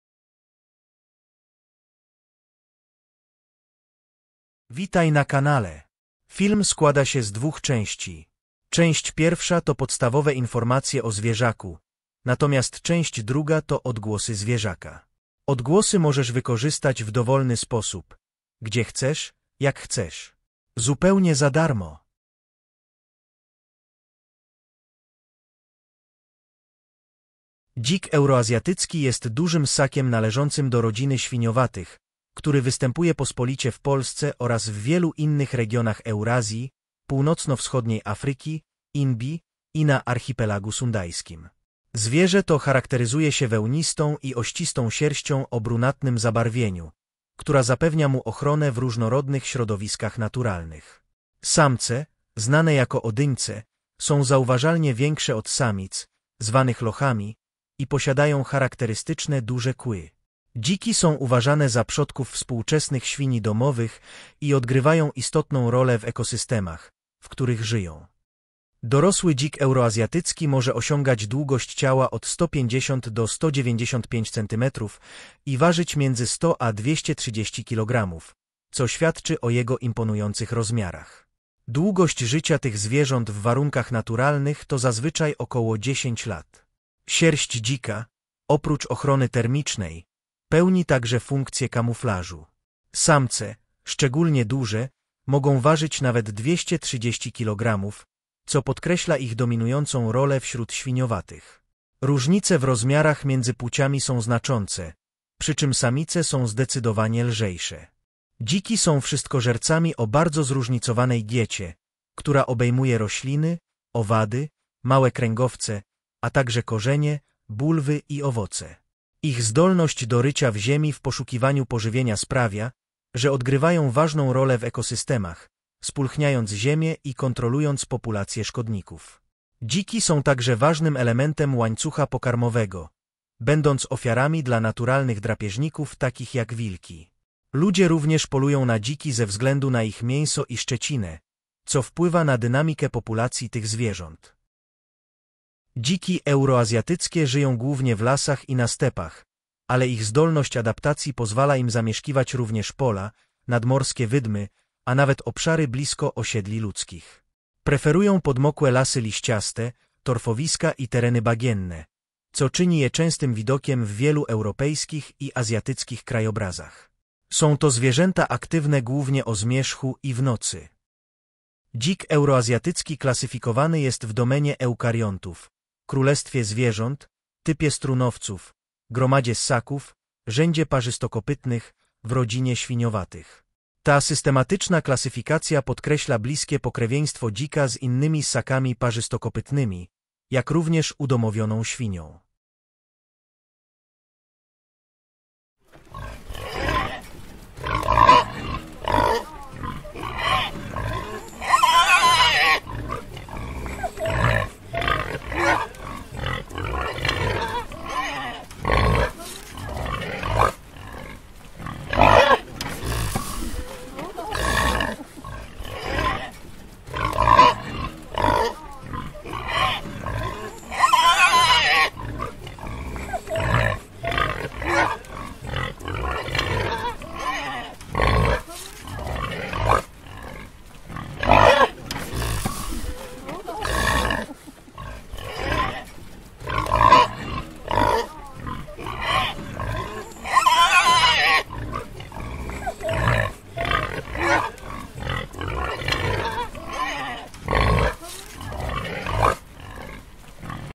Odgłosy dzika, informacje, ciekawostki🐗 chrumkanie dzika w lesie 🐗
Odgłosy dzika, informacje, ciekawostki🐗 chrumkanie dzika w lesie 🐗 Jak robi dzik?

00:00 Wstęp
00:25 Informacje o dziku
3:15 Odgłosy dzika

Odgłosy dzika w lesie, jak robi dzik oraz informacje i ciekawostki o dzikach 🐗

Dzik euroazjatycki jest dużym ssakiem należącym do rodziny świniowatych, który występuje pospolicie w Polsce oraz w wielu innych regionach Eurazji, północno-wschodniej Afryki, Indii, i na Archipelagu Sundajskim. Zwierzę to charakteryzuje się wełnistą i ościstą sierścią o brunatnym zabarwieniu, która zapewnia mu ochronę w różnorodnych środowiskach naturalnych. Samce, znane jako odyńce, są zauważalnie większe od samic, zwanych lochami, i posiadają charakterystyczne duże kły. Dziki są uważane za przodków współczesnych świni domowych i odgrywają istotną rolę w ekosystemach, w których żyją.

Dorosły dzik euroazjatycki może osiągać długość ciała od 150 do 195 cm i ważyć między 100 a 230 kg, co świadczy o jego imponujących rozmiarach. Długość życia tych zwierząt w warunkach naturalnych to zazwyczaj około 10 lat. Sierść dzika, oprócz ochrony termicznej, pełni także funkcję kamuflażu. Samce, szczególnie duże, mogą ważyć nawet 230 kg, co podkreśla ich dominującą rolę wśród świniowatych. Różnice w rozmiarach między płciami są znaczące, przy czym samice są zdecydowanie lżejsze.

Dziki są wszystkożercami o bardzo zróżnicowanej diecie, która obejmuje rośliny, owady, małe kręgowce, a także korzenie, bulwy i owoce. Ich zdolność do rycia w ziemi w poszukiwaniu pożywienia sprawia, że odgrywają ważną rolę w ekosystemach, spulchniając ziemię i kontrolując populacje szkodników. Dziki są także ważnym elementem łańcucha pokarmowego, będąc ofiarami dla naturalnych drapieżników takich jak wilki. Ludzie również polują na dziki ze względu na ich mięso i szczecinę, co wpływa na dynamikę populacji tych zwierząt.

Dziki euroazjatyckie żyją głównie w lasach i na stepach, ale ich zdolność adaptacji pozwala im zamieszkiwać również pola, nadmorskie wydmy, a nawet obszary blisko osiedli ludzkich. Preferują podmokłe lasy liściaste, torfowiska i tereny bagienne, co czyni je częstym widokiem w wielu europejskich i azjatyckich krajobrazach. Są to zwierzęta aktywne głównie o zmierzchu i w nocy.

Dzik euroazjatycki klasyfikowany jest w domenie Eukariontów, królestwie Zwierząt, typie Strunowców, gromadzie Ssaków, rzędzie Parzystokopytnych, w rodzinie Świniowatych. Ta systematyczna klasyfikacja podkreśla bliskie pokrewieństwo dzika z innymi ssakami parzystokopytnymi, jak również udomowioną świnią.